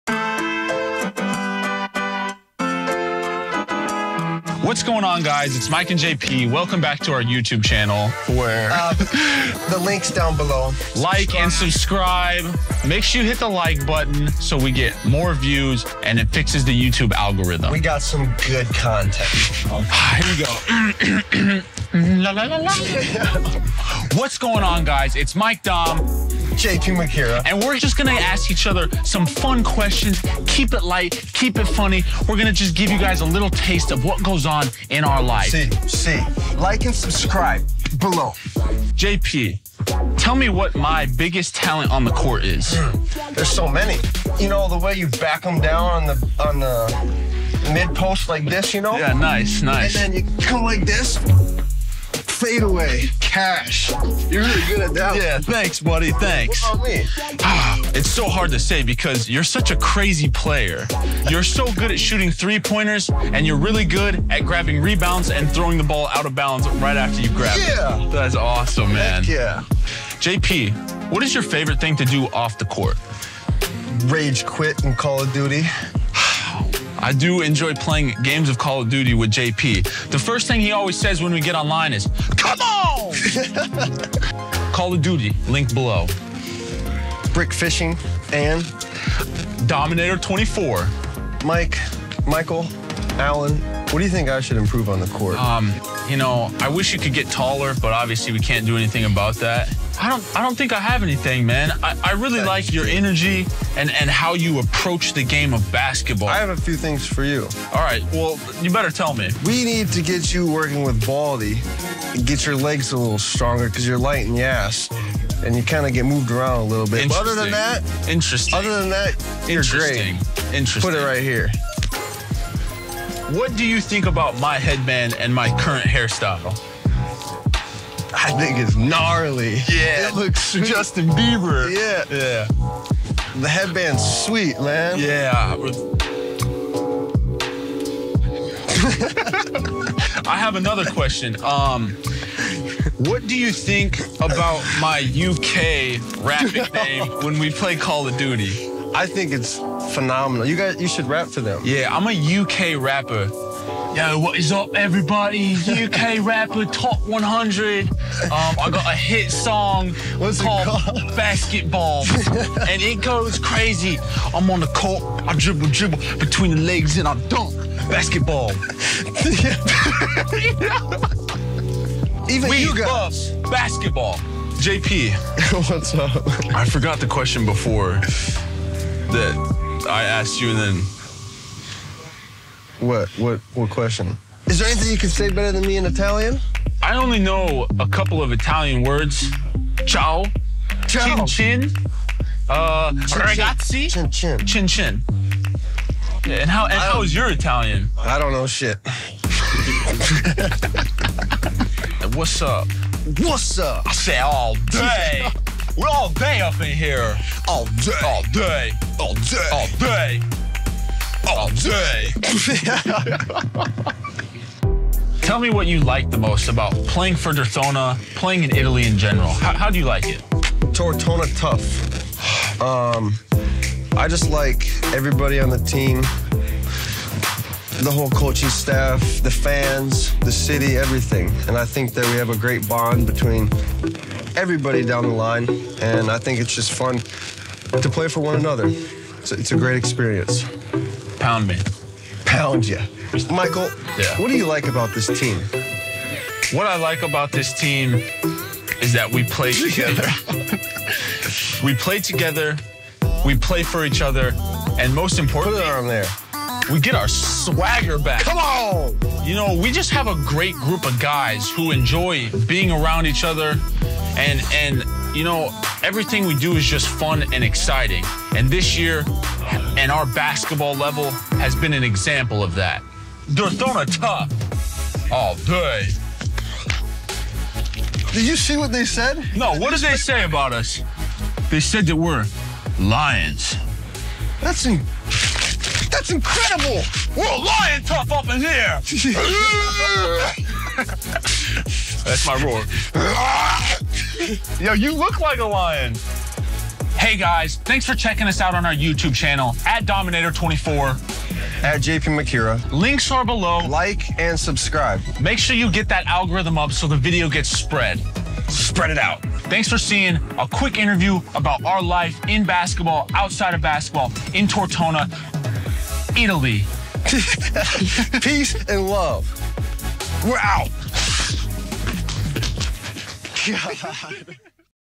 JP Macura e Mike Daum
🎬 JP e Dauminator, una sola parola: 𝙀𝙎𝙋𝙇𝙊𝙎𝙄𝙑𝙄 💥
Con una coppia così le risate sono assicurate 😂

Questo è il Canale YouTube ufficiale della Legabasket Serie A! 🏀
Ogni settimana trovi gli highlights delle partite, interviste speciali e tanti altri contenuti.